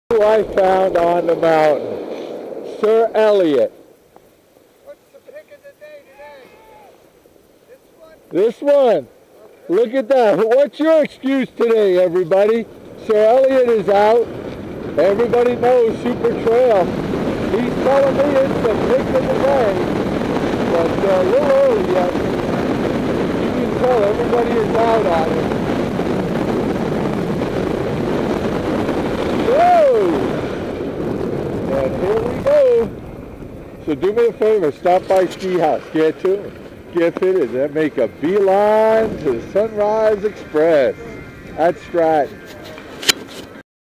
Feb 6th The Big Man catches Sir Elliot carving up Sunrise Supertrail.
Sir Elliot "Pick of the Day".  And whats your excuse for not skiing Stratton Mtn.